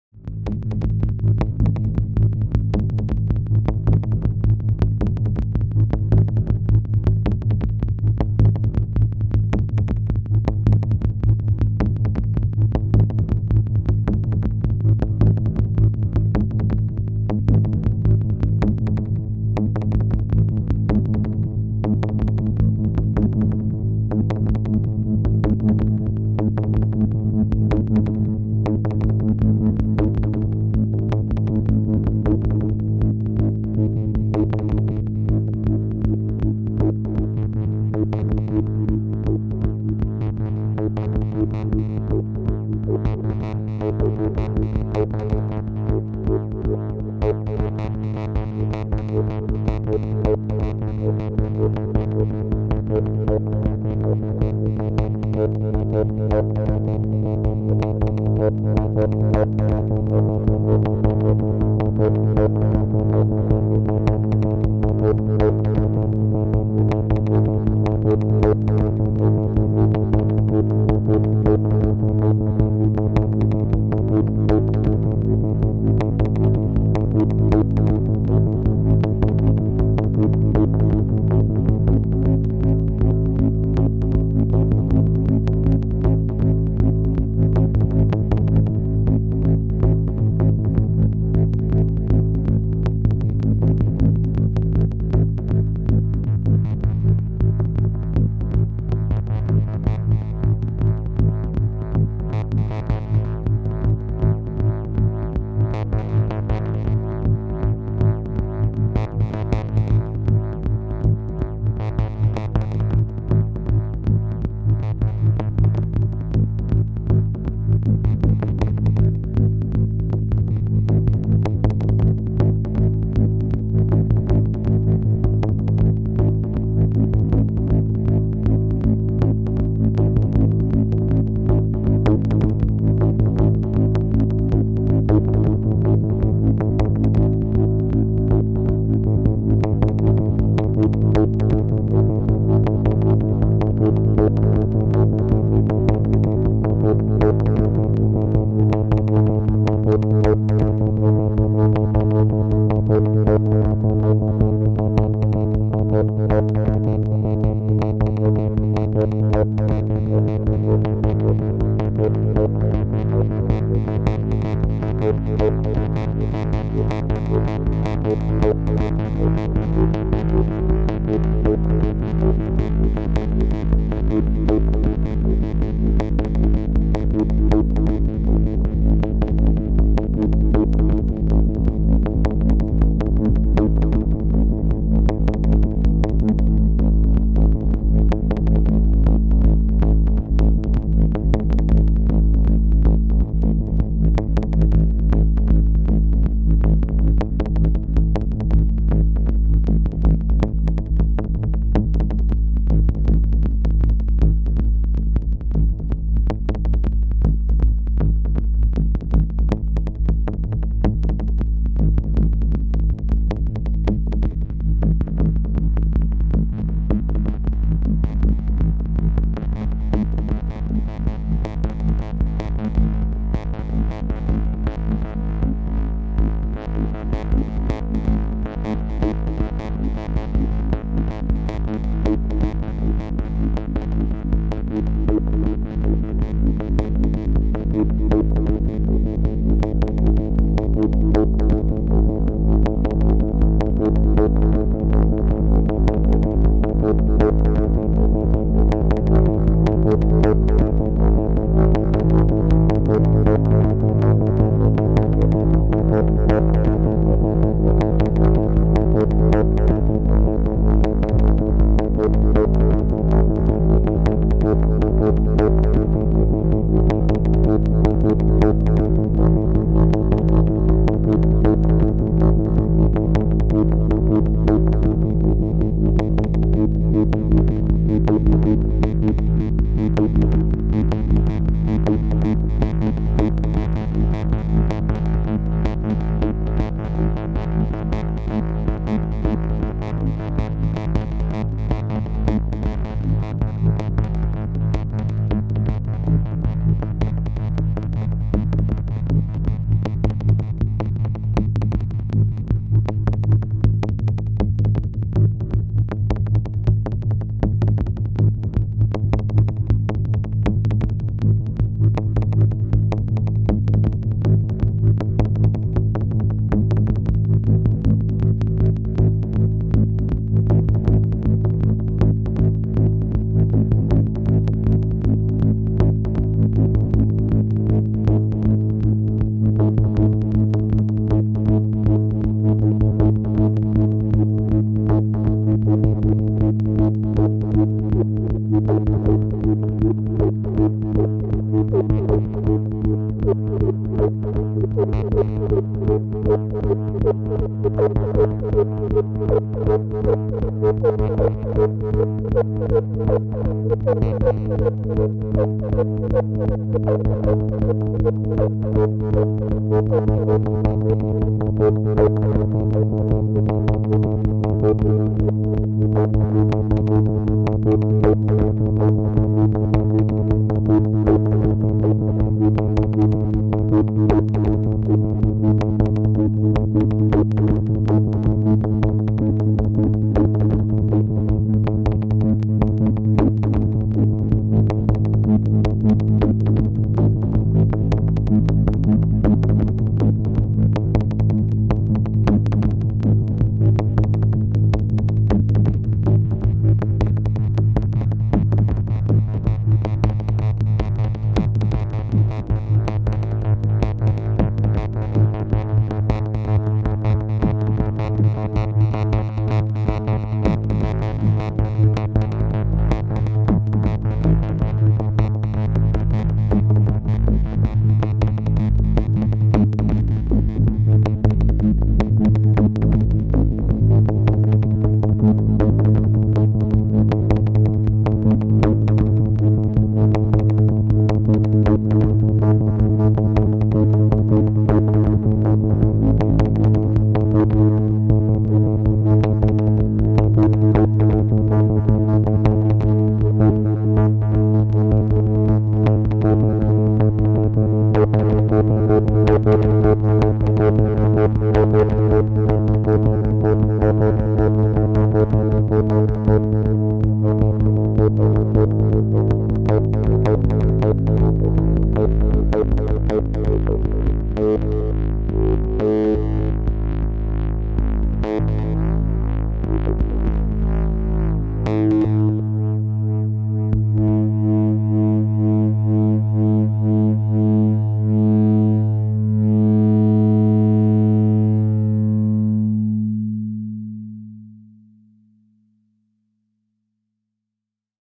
Slippy – 60hp of techno(ish) eurorack fun.
Putting the mini system through its paces.

Modules are mostly left over modules from my other system (7u).

The whole thing is USB powered and fits nicely on my lap.